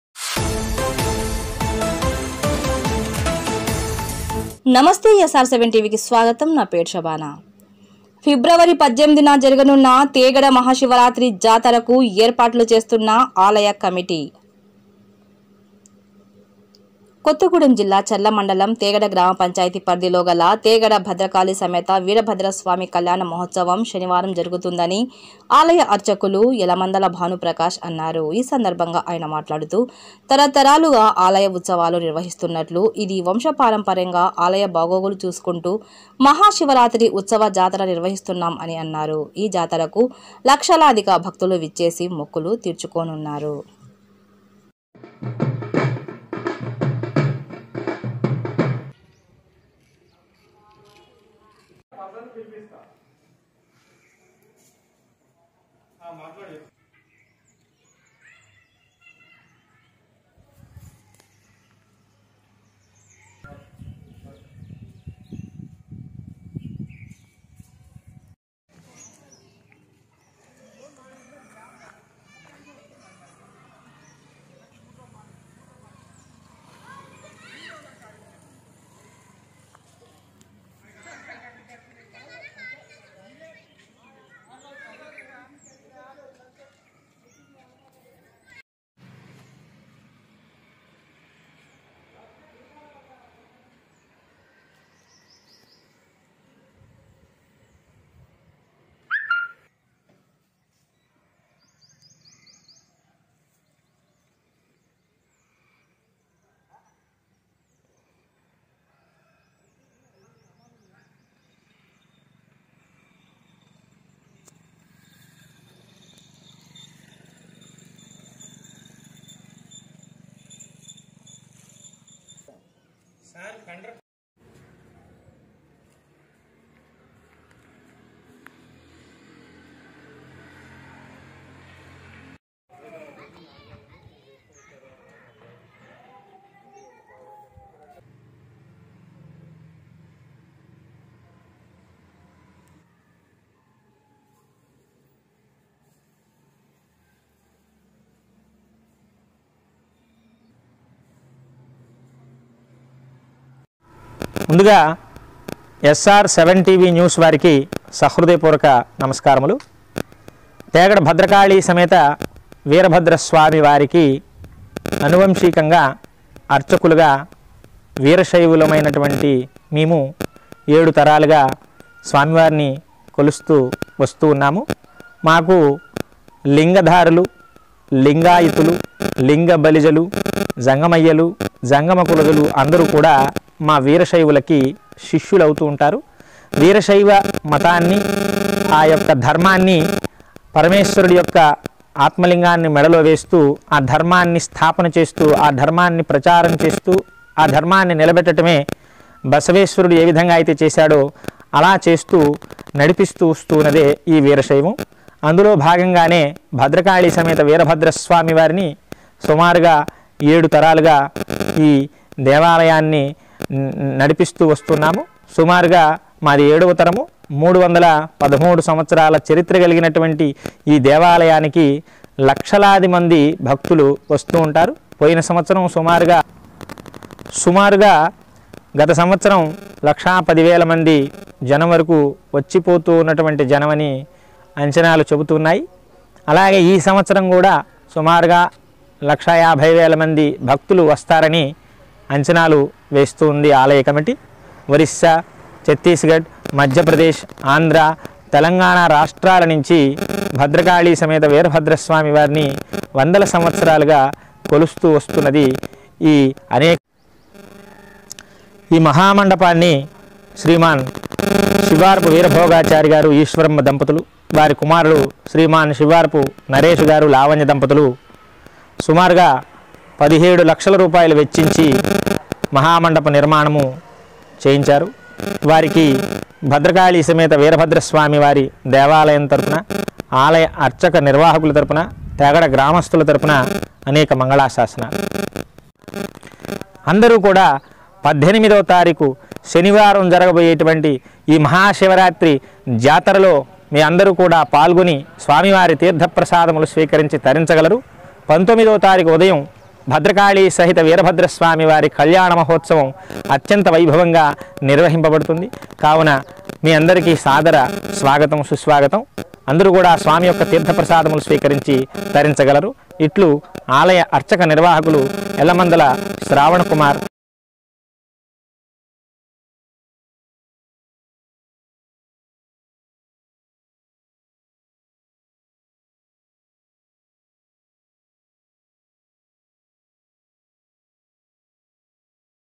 చర్ల మండలంలో ఫిబ్రవరి 18న జరగనున్న తేగడ మహాశివరాత్రి జాతరకు ఏర్పాట్లు చేస్తున్న ఆలయ కమిటీ
*ఫిబ్రవరి 18 న జరగనున్న తేగడ మహాశివరాత్రి జాతరకు ఏర్పాట్లు చేస్తున్న ఆలయ కమిటీ*

కొత్తగూడెం జిల్లా చర్ల మండలం తెగడ గ్రామపంచాయతీ పరిధిలోగల తేగడ భద్రకాళి సమేత వీరభద్రస్వామి కళ్యాణ మహోత్సవం శనివారం జరుగుతుందని ఆలయ అర్చకులు ఎలమందల బాను ప్రకాష్ అన్నారు , ఈ సందర్భం గా ఆయన మాట్లాడుతూ తర తరాలుగా ఆలయం ఉత్సవాలు నిర్వహిస్తున్నట్లు ఇది వంశపర్యపరంగా ఆలయ బాగోగులు చూసుకుంటూ   మహాశివరాత్రి ఉత్సవ జాతర నిర్వహిస్తున్నాం అని అన్నారు .ఈ జాతరకు లక్షలాదిగా భక్తులు విచ్చేసి మొక్కులు తీర్చుకోన్నారు